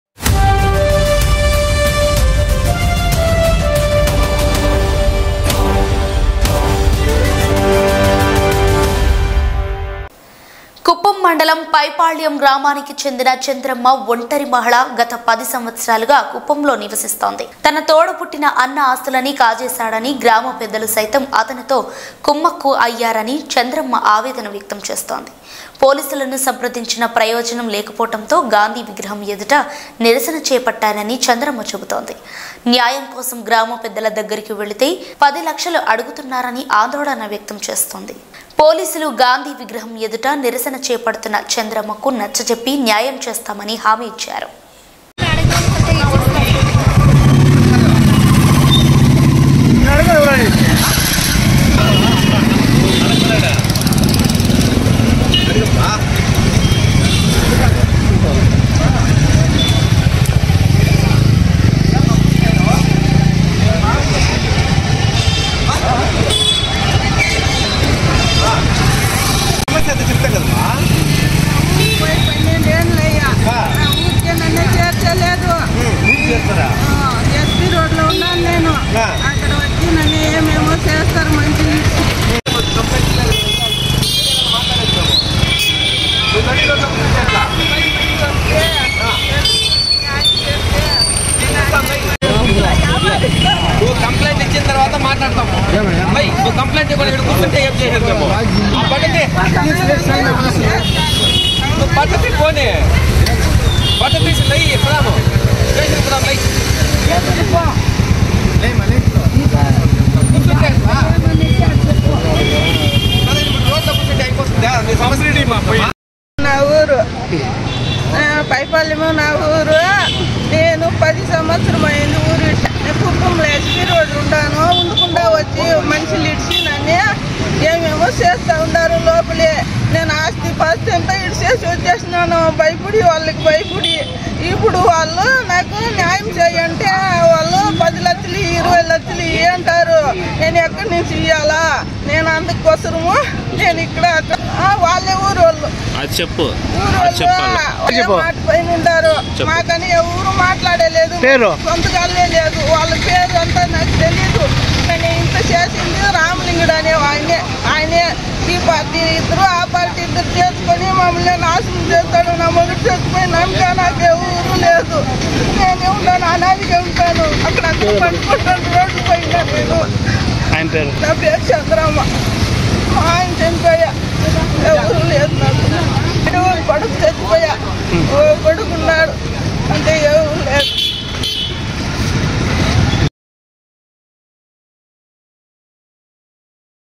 తన తోడబుట్టిన అన్న ఆస్తులన్ని కాశాజేశాడని.... చెల్లెలు చంద్రమ్మ
చిత్తూరు జిల్లా..

కుప్పం..
చెల్లెల్లి ఆస్తి కాజేసిన అన్న.
కుప్పం మండలం పైపాళ్యం గ్రామానికి చెందిన చంద్రమ్మ ఒంటరి మహిళ గత పది సంవత్సరాలుగా కుప్పంలో నివసిస్తోంది. తన తోడబుట్టిన అన్న ఆస్తులన్ని కాశాజేశాడని, గ్రామ పెద్దలు సైతం అతనితో కుమ్మక్కు అయ్యారని చంద్రమ్మ ఆవేదన వ్యక్తం చేస్తోంది. పోలీసులను సంప్రదించినా ప్రయోజనం లేకపోవడంతో గాంధీ విగ్రహం ఎదుట నిరసన చేపట్టానని చంద్రమ్మ చెబుతోంది. న్యాయం కోసం గ్రామ‌ పెద్దల దగ్గరికి వెలితే పది లక్షలు అడుతున్నారని ఆందోళన వ్యక్తం చేస్తోంది‌. పోలీసులు గాంధీ విగ్రహం ఎదుట నిరసన చేపడతున్న చంద్రమ్మకు నచ్చచెప్పి న్యాయం చేస్తామని హామీ ఇచ్చారు...